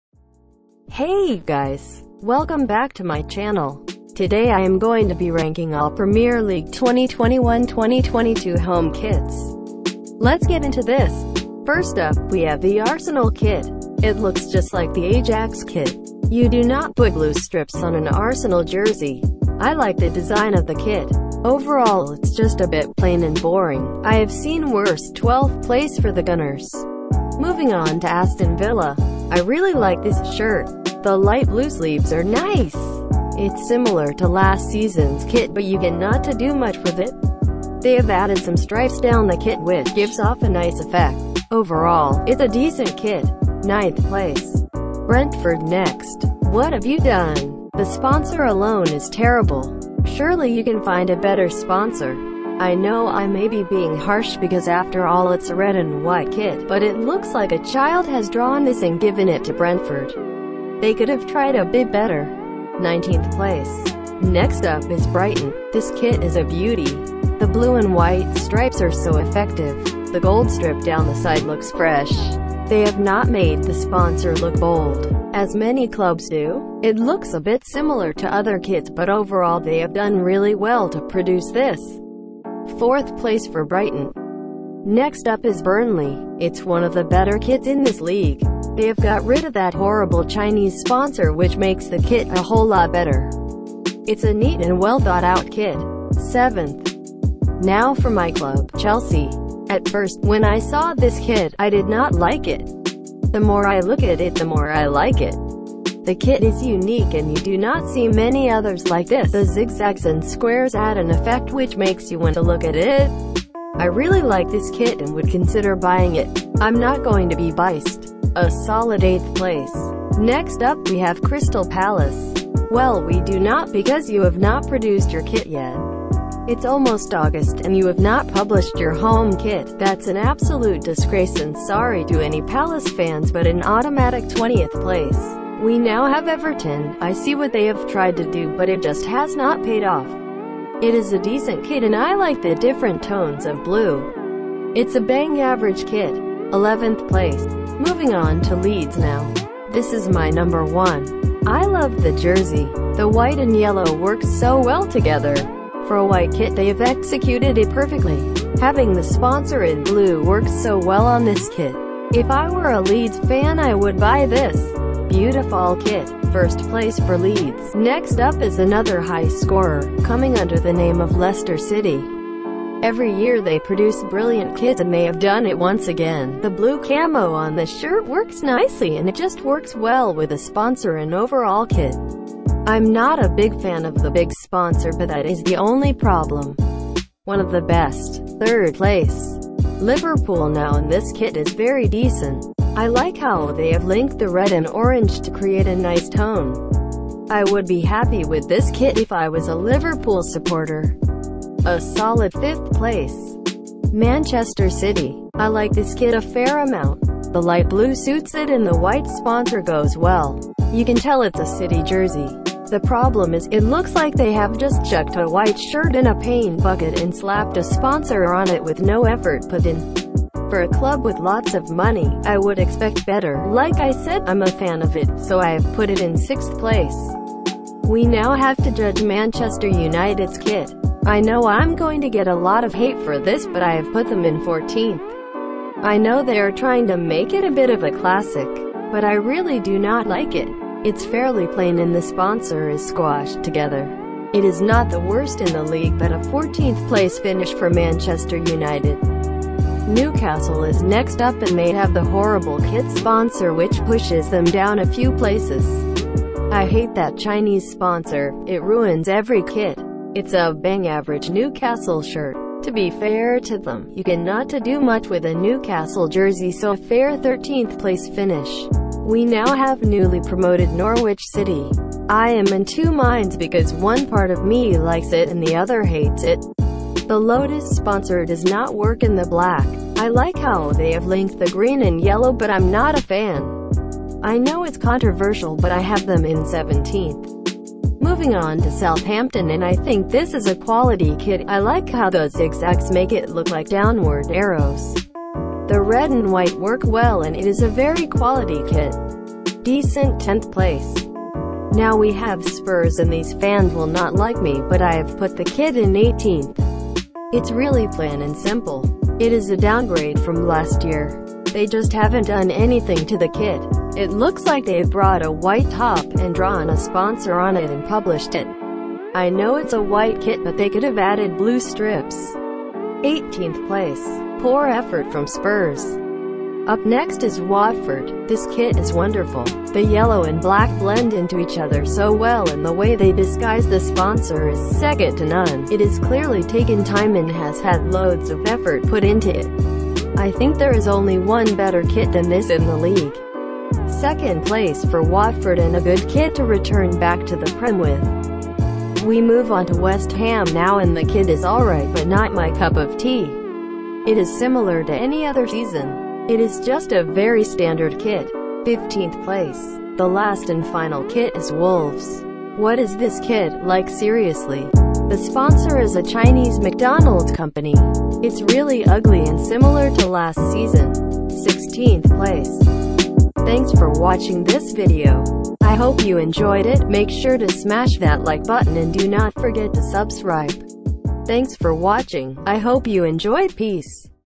Ranking EVERY 21/22 Premier League Home kit!!! *controversial*⚽️😱👀
Hey guys!
This is my premier league 21/22 Home kit rankings!
Peace! ⚽️